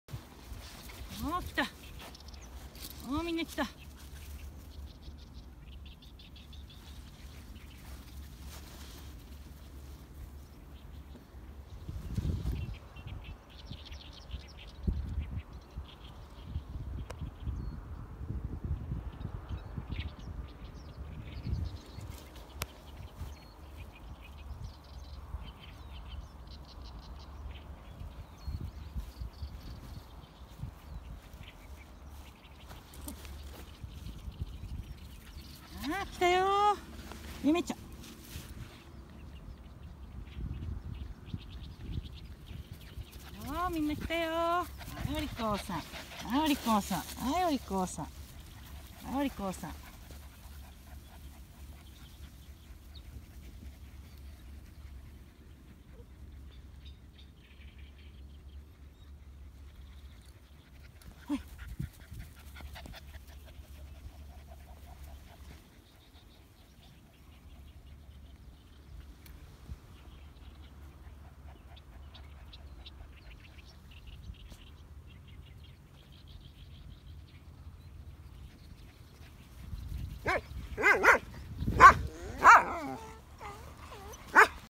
秋田犬 純 天、生後2ヶ月と1日の仔犬達
ワクチン打った当時と翌日はおとなしく過ごし、晴れたので少し発散に。
ノミ・ダニの薬も使ったので、草むらにも行けるが、石を食べるので要注意。